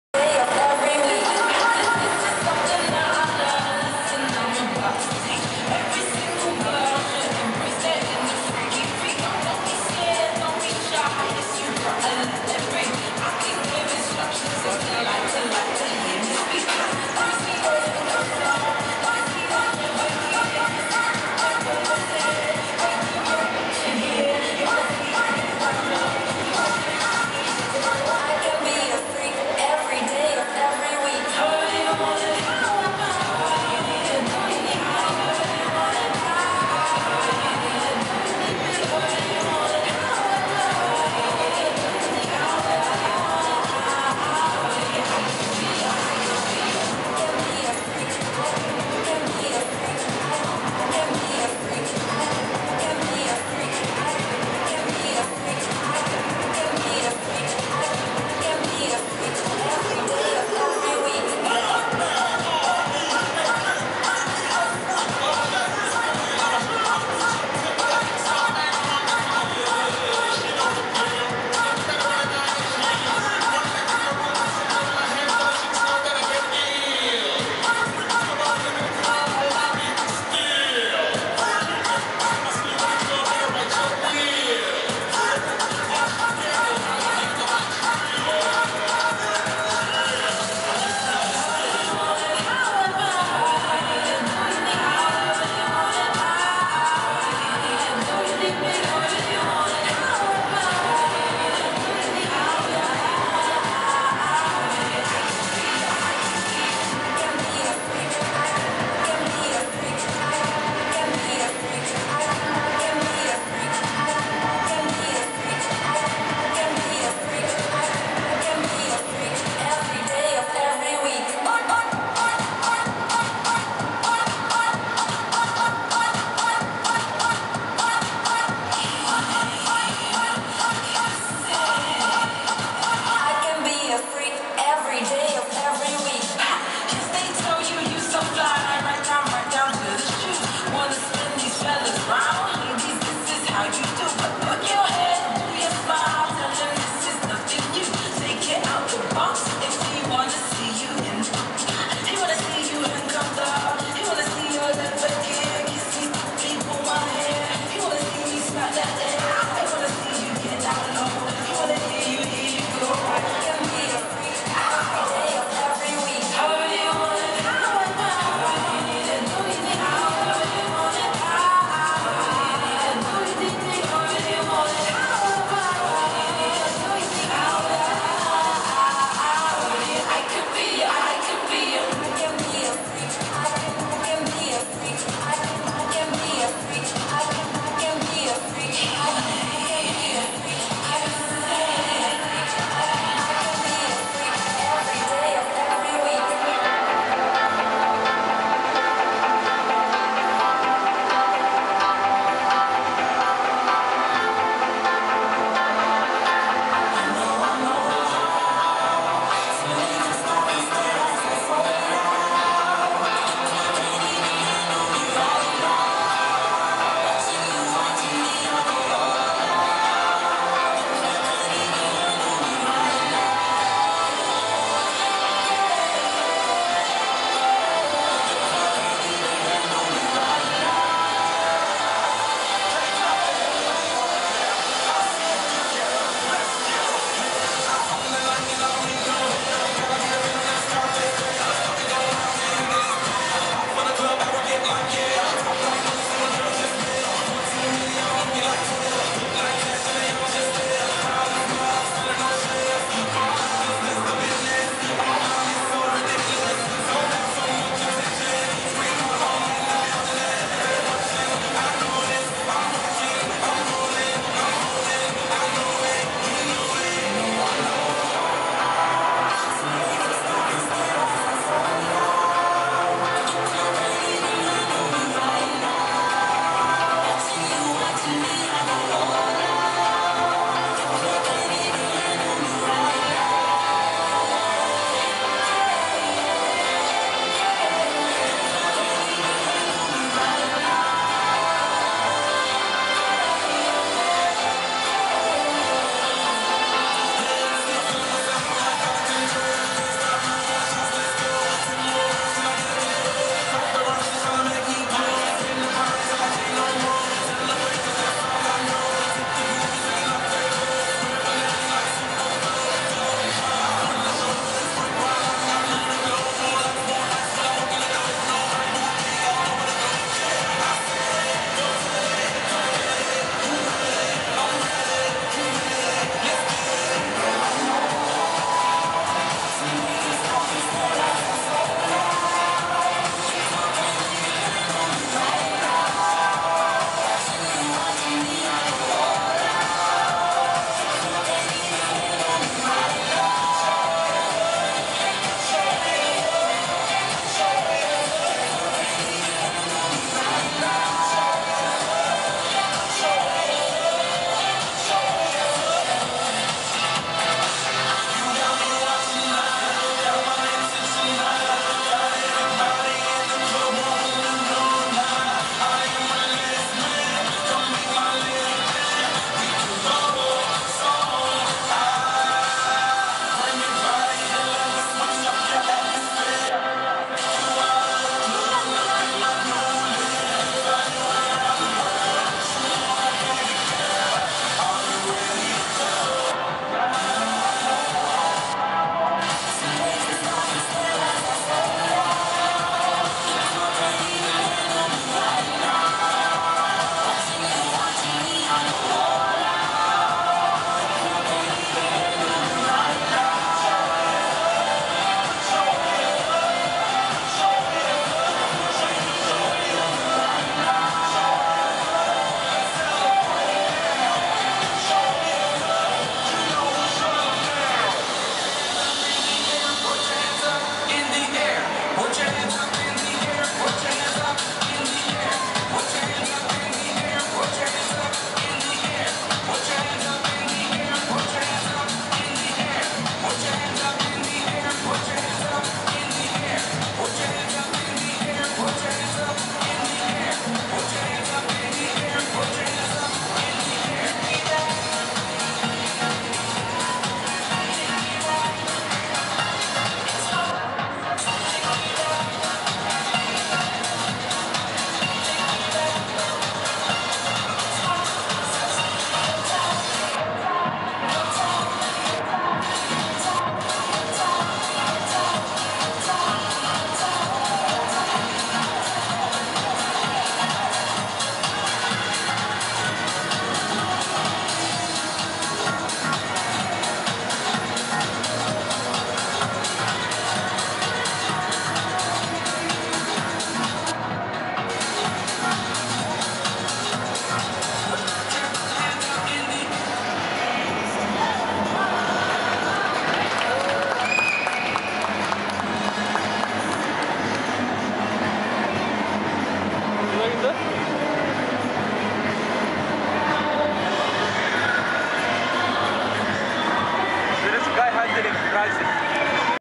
dubai festival city gaming exibition 2010.wmv
this is a recording of the gaming exibition in dubai festival city 2010